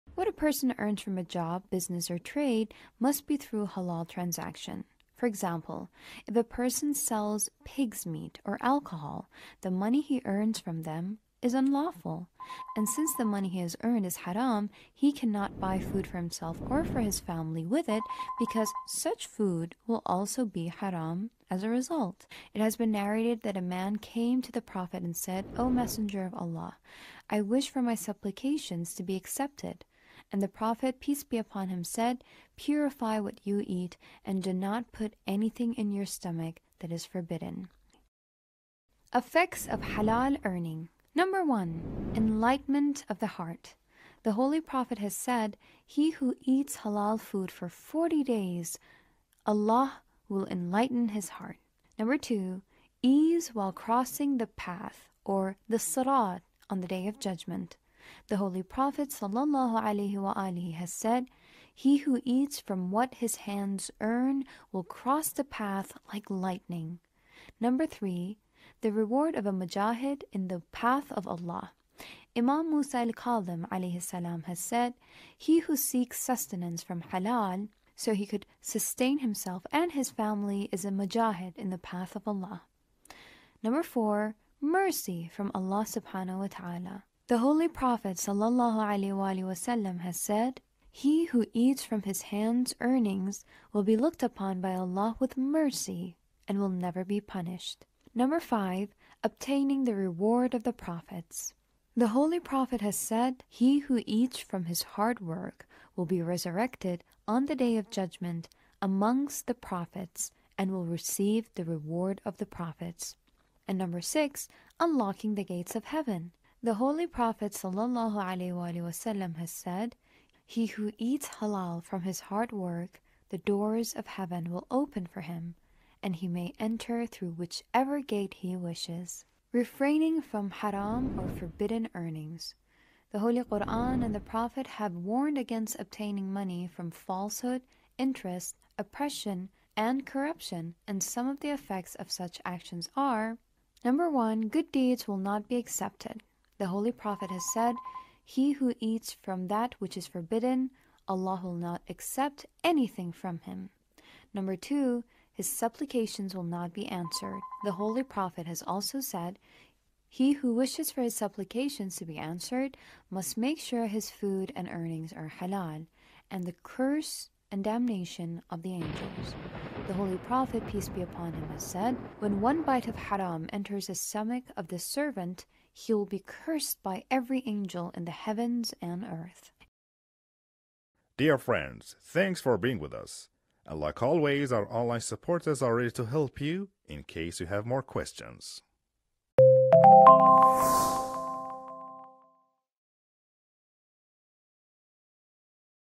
Why Islam Emphasizes so Much on Halal Earnings?
Have you ever wondered why Islam places such a strong emphasis on earning through Halal means? Join us in this enlightening video as we delve deep into the fundamental principles behind the importance of Halal earnings in Islam.
In Islam, the concept of Halal earnings goes beyond mere financial transactions; it encompasses ethics, morality, and the overall well-being of individuals and society. This video will explore the profound reasons why Islam places such a great emphasis on ensuring that one's earnings are Halal.

00:00 | Intro
00:39 | Effects on halal earnings

Eager to learn more about this topic? Join our course and become a master in no time! ⬇️⬇️⬇️⬇️⬇️⬇️⬇️⬇️⬇️⬇️⬇️⬇️⬇️